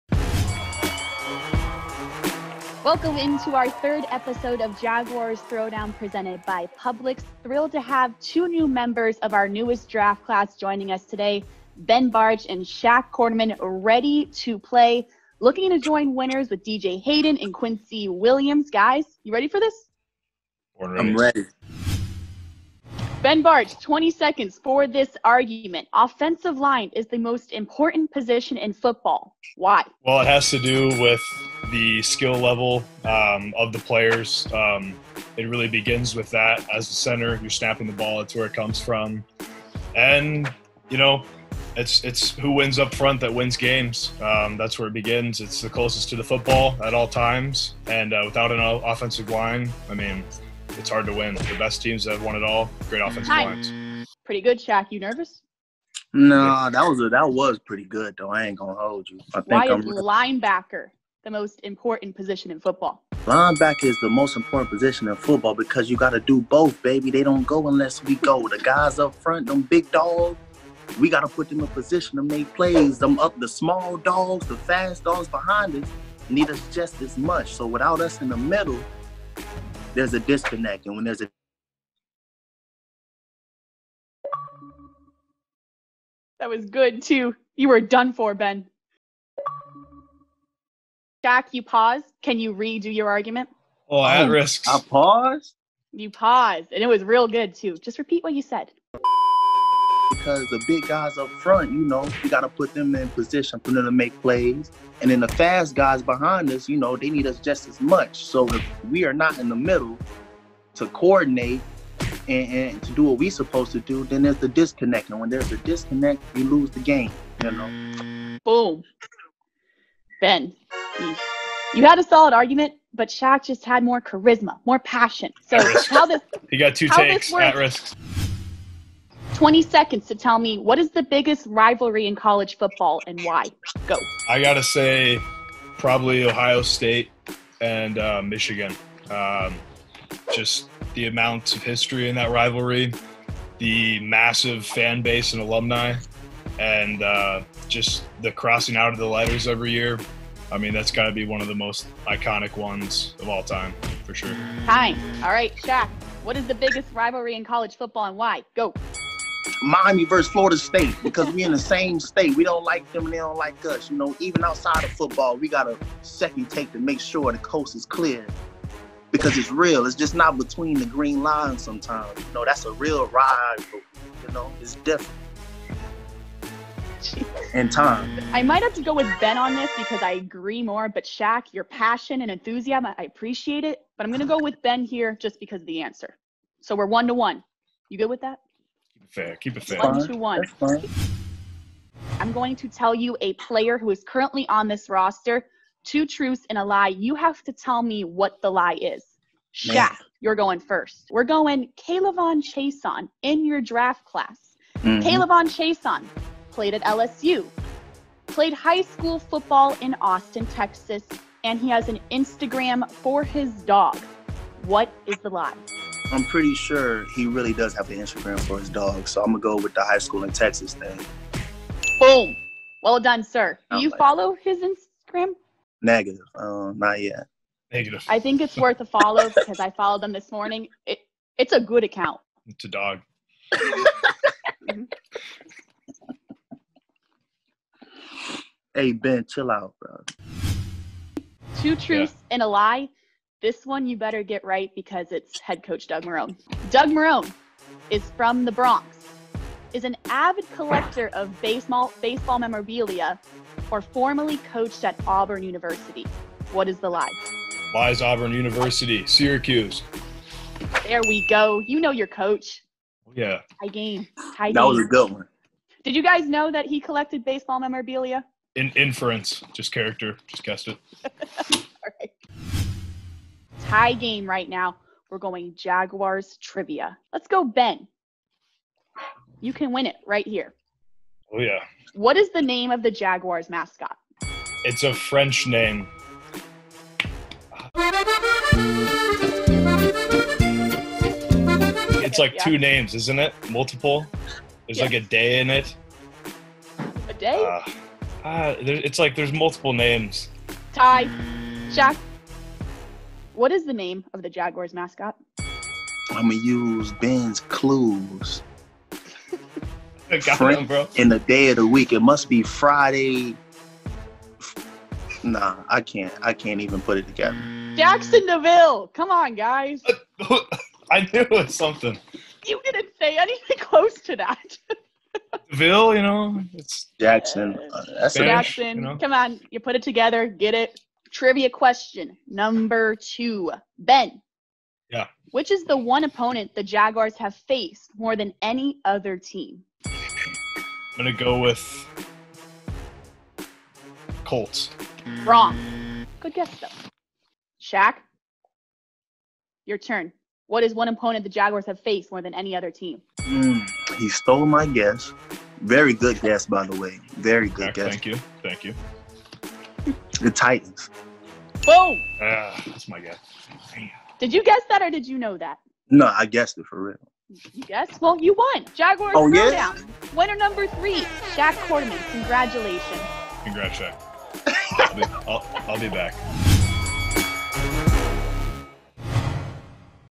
Jaguars Throwdown (Ep. 3): Ben Bartch vs. Shaq Quarterman
Rookies Ben Bartch and Shaq Quarterman battle if offensive lineman or linebackers are more important, the biggest rivalry in college football, and answer trivia about their teammates.